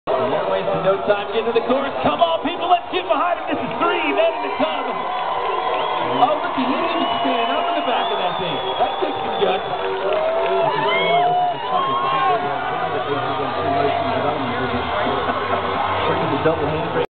Red Bull Soapbox Derby Dallas 2012
Team: 3 men in a tub. These guys were great. I wasnt able to record their crash but they both had some serious road rash.